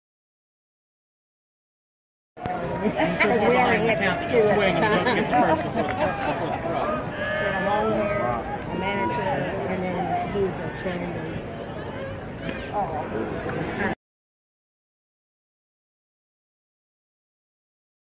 Louisville Cardinals Playing Illinois State in the Regional (HD)
Final game of the Regional. Louisville wins 4-3 with a walk off single. It was a great game. Very exciting. Great weather. Great crowd. Took the Mrs. and she talked to the woman next to her the whole game. Our seats were perfect too. We were in shade 90% of the time. Go Cards in the Super Regionals this weekend. This was on 3-Jun-19